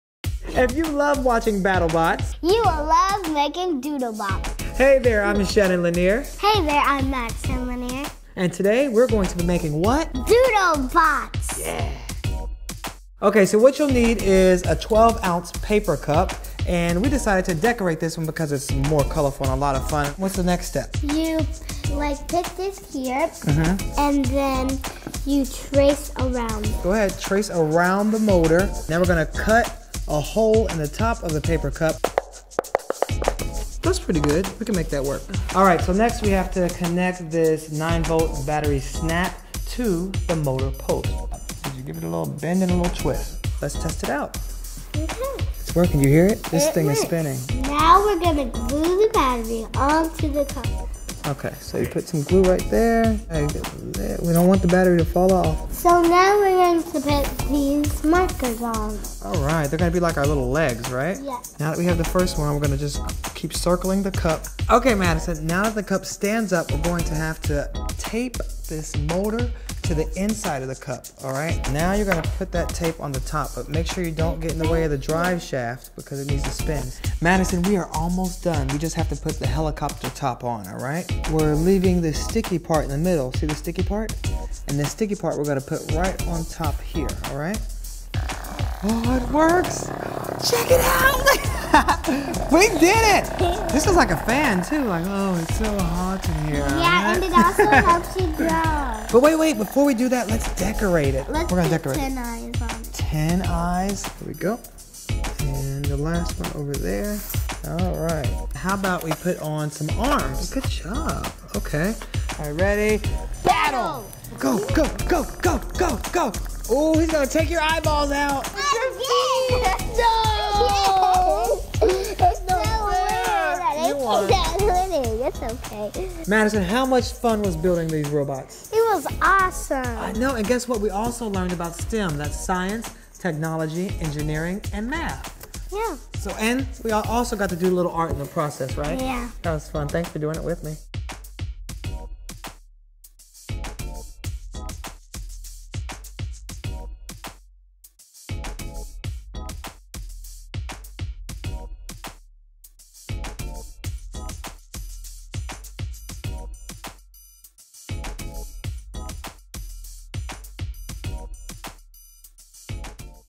BattleBots: How to Build a Battling Doodle Bot
BattleBots is back on TV. If you’re like us, you’re stoked. Because watching two robots smash into each other is pretty damn fun. Beyond the visceral thrills, however, these killer robots are great examples of the possibilities and excitement of science, technology, engineering, and mathematics.

To celebrate the show’s Science Channel update with another entry in our “Maker Dads” series, which will be airing during the show. This week, Shannon LaNier and his daughter Madison show you how to build a simple doodle robot.

What you'll need to build a doodle bot:
- 9V Battery
- A wired snap for a 9V battery
- A 12-oz paper cup
- A 3-9 V electric motor with 2.3mm shaft diameter
- 4 thin markers
- A roll of duct tape
- 2 rolls of colorful duct tape
- A pencil
- A box cutter or knife
- 2 popsicle sticks
- Googly eyes
- Super glue

Note: We may earn $ if you buy using those links!